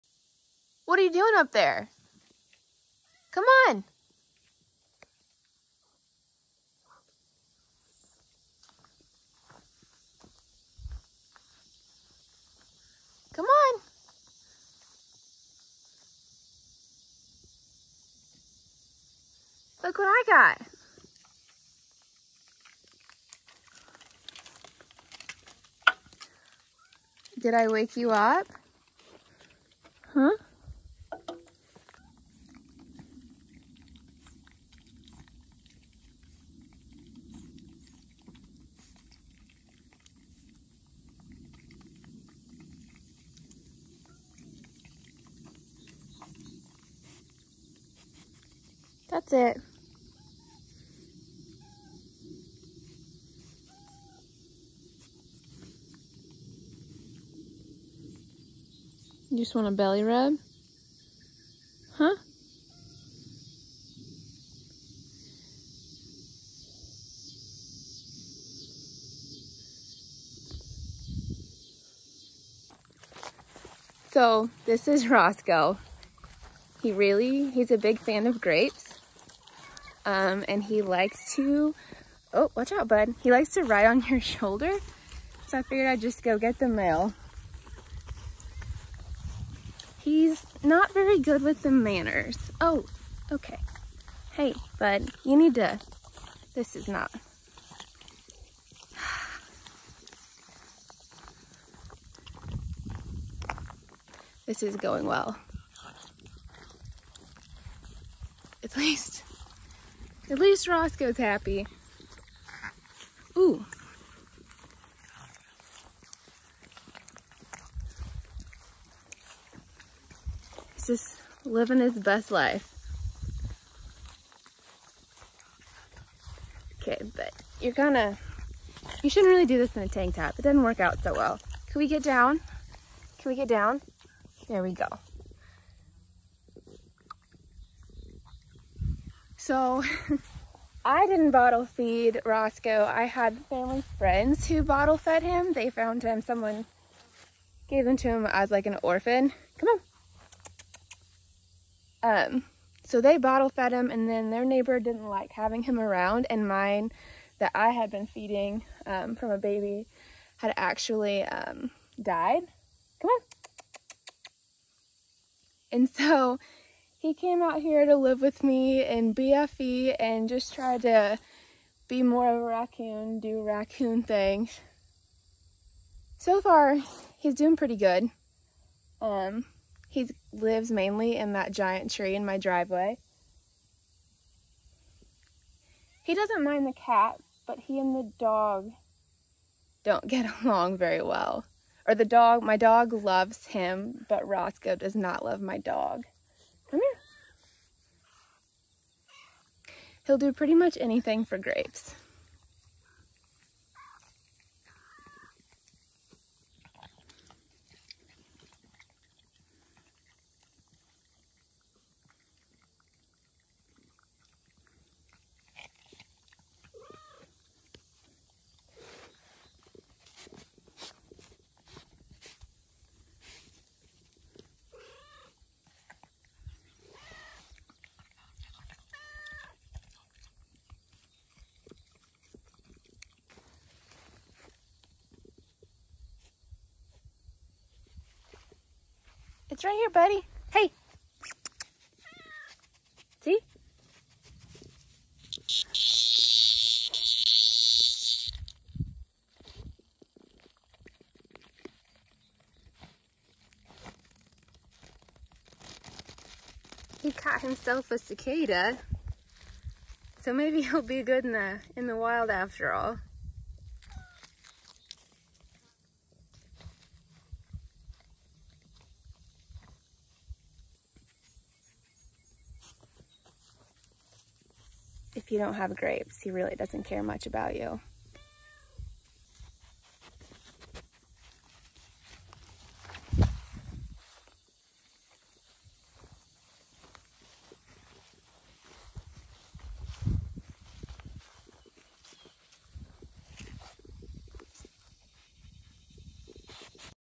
Meet Roscoe the Raccoon
This is everyone’s favorite little a**hole, Roscoe. He was rescued and bottle fed by family friends and then came to live with me in the middle of nowhere while he figures out how to be a raccoon. He’s gotten pretty good at finding his own food, but his chunky little butt will never turn anything you have to give him down.

Facebook - Heart of the Midwest